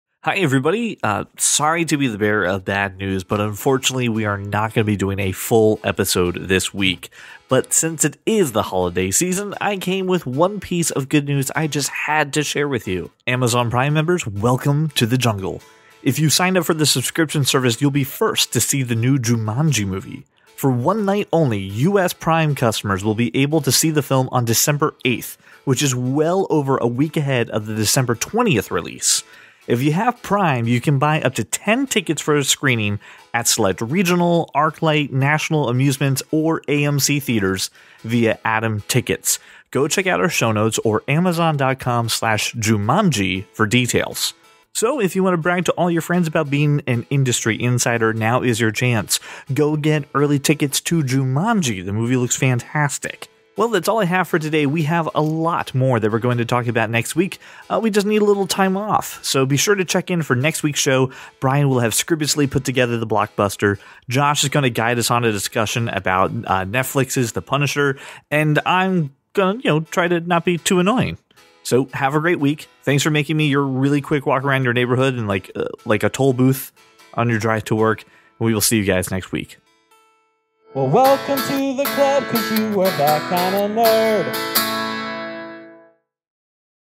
Nerd Byte 12/4
For one night only, US Prime subscribers will be able to see the film on 8 December, well over a week ahead of the December 20th general release.
If you have Prime you can buy up to 10 tickets for screenings at selected Regal, ArcLight, National Amusements or AMC theaters via Atom Tickets — check out for details

 Share your opinion or a story from your nerdy life.